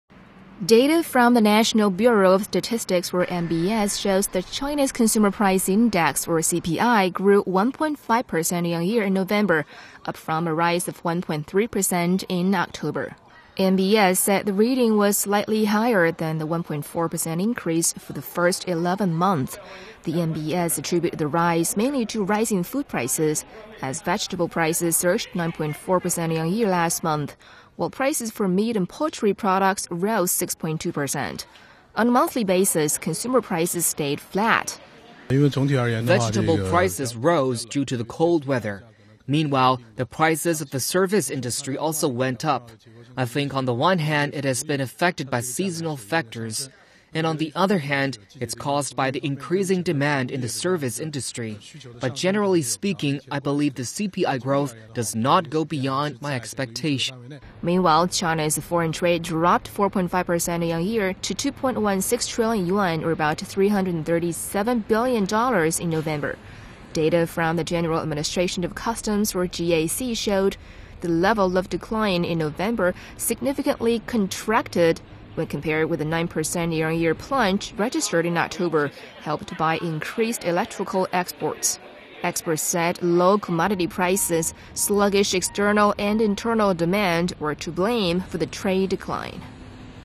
China's November inflation grows 1.5 pct
Data from the National Bureau of Statistics (NBS) shows that China's consumer price index grew 1.5 percent year on year in November, up from a rise of 1.3 percent in October.

NBS said The reading was slightly higher than a 1.4-percent increase for the first 11 months.

The NBS attributed the rise mainly to rising food prices, as vegetable prices surged 9.4 percent year on year last month, while prices for meat and poultry products rose 6.2 percent.

On a monthly basis, consumer prices stayed flat.

SOUNDBITE:  SHAO YU  Orient Securities

"Vegetable prices rose due to the cold weather. Meanwhile, the prices of the service industry also went up. I think on the one hand it has been affected by seasonal factors, on the other hand, it's caused by the increasing demand in the service industy. But generally speaking, I believe the CPI growth does not go beyond my expectations."

Meanwhile,  China's foreign trade dropped 4.5 percent year on year to 2.16 trillion yuan (337 billion U.S. dollars) in November.

Data from the General Administration of Customs (GAC) showed that the level of decline in November significantly contracted when compared with the 9 percent year on year plunge registered in October, helped by increased electrical exports.

Expert said low commodity prices, sluggish external and internal demand were to blame for the trade decline.